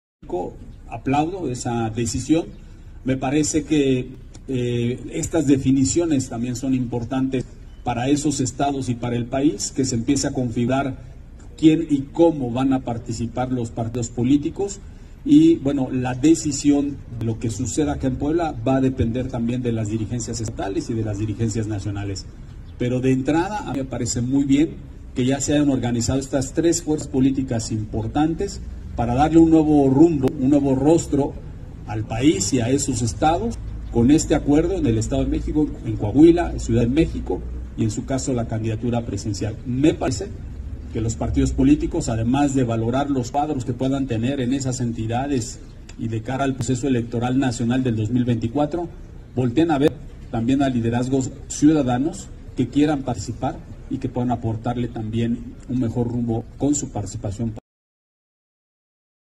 Eduardo Rivera aplaudió la alianza entre el PAN-PRI-PRD de cara al proceso electoral de 2024
De cara al proceso electoral de 2024, el presidente municipal Eduardo Rivera, aplaudió la alianza conformada entre el PAN-PRI-PRD "para darle un nuevo rumbo al país y a esos estados".